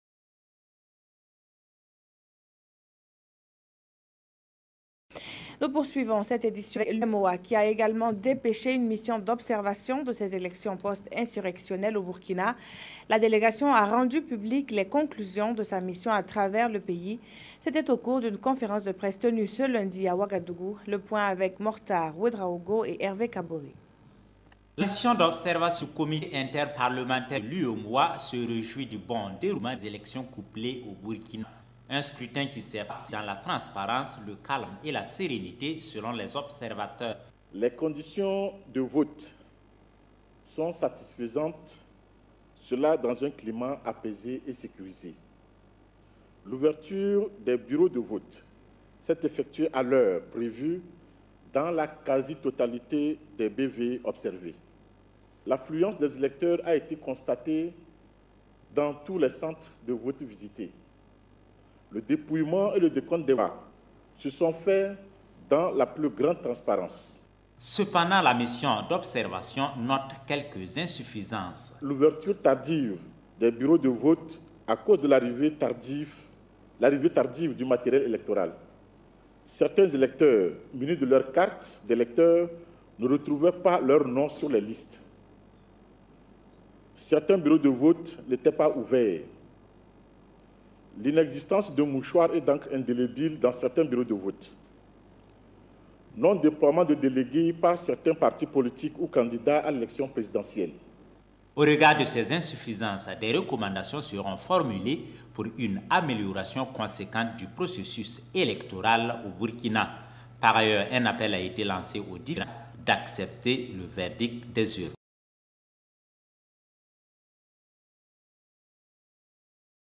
Conférence de presse de la délégation des missions d'observation de l'UEMO
Conference de presse de la délégation des missions d'observation de l'UEMOA pour rendre public les conclusions de sa mission